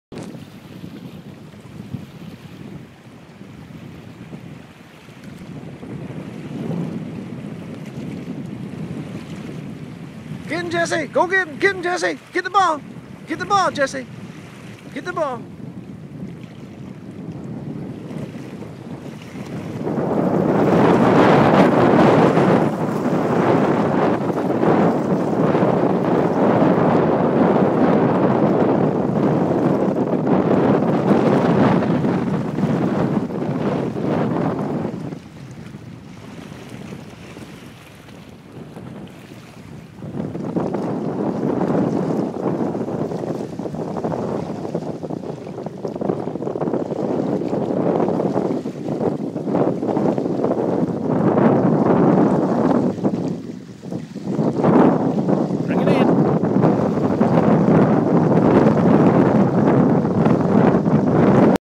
Jesse's first swin
Our Italian Spinone Jesse have her first swim.